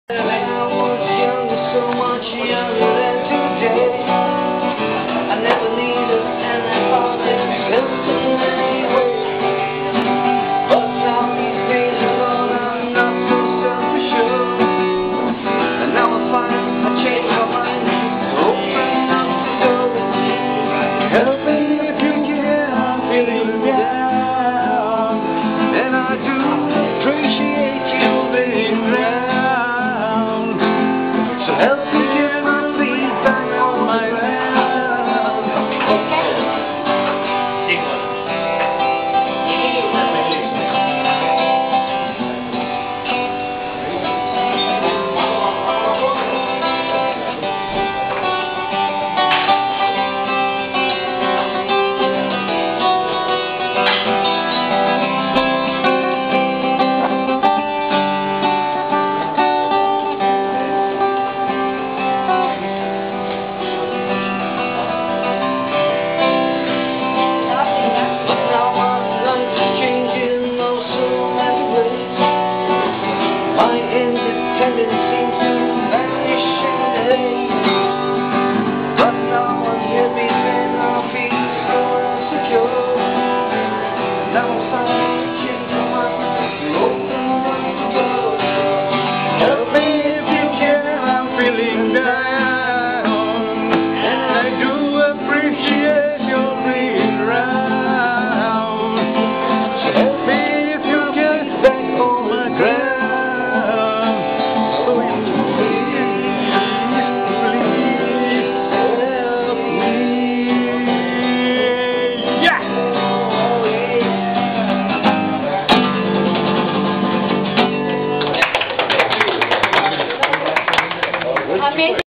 Songs for Krishna by Glen Shea 5
Glen Shea singing at Big Max, Kolkata, India, on 27th, december, 2010, for the Reunion for Krishna our friend from AG School since 1974 in Kolkata, India and now She lives in Bangalore.